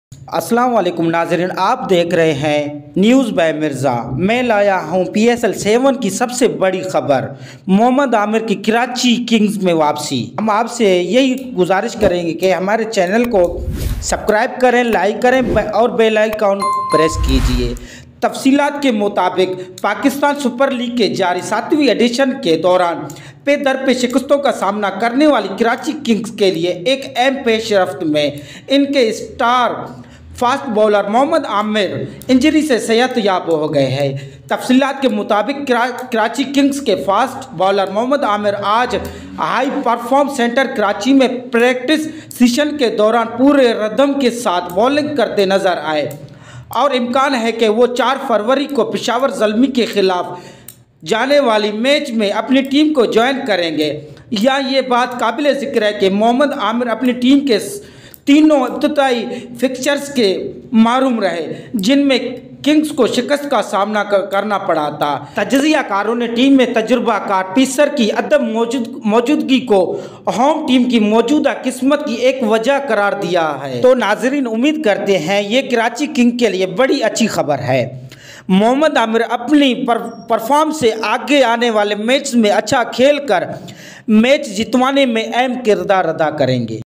karachi king ke liye bari khabar
good news by karachi king mohamnad amir ki karachi king me entery